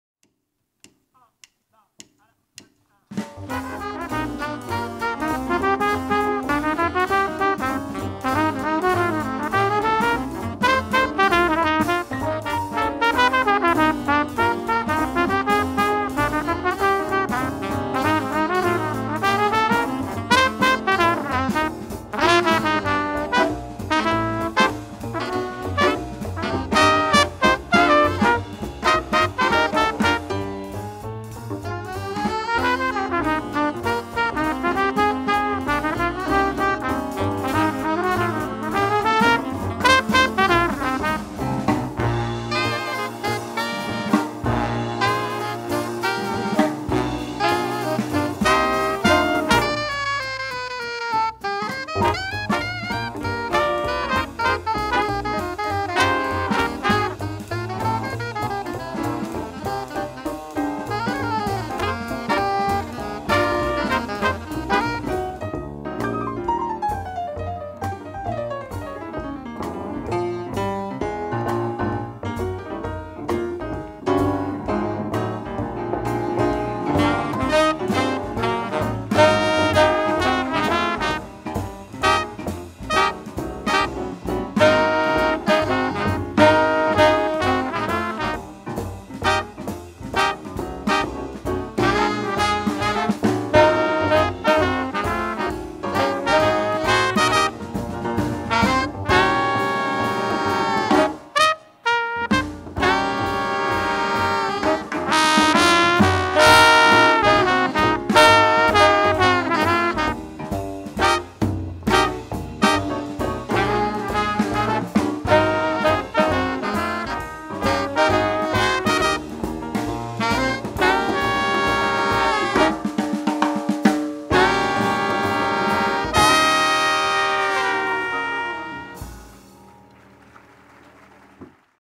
La Vella Dixieland al Pati de Can Xifré
La Vella Dixieland al Pati de Can Xifré d'Arenys de Mar el passat divendres dia 23 de juliol del 2010.